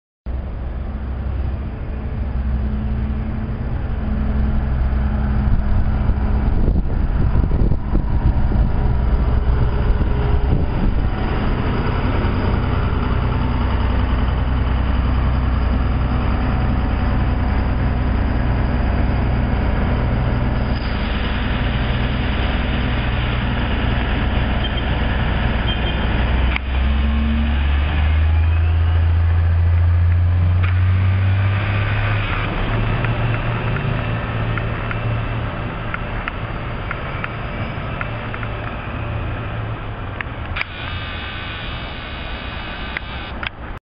MaBSTOA: Old Gen Orion VII # 6707 On A VA Hospital-bound M9
Taken on 7.10.10 at East 14th Street & Avenue C.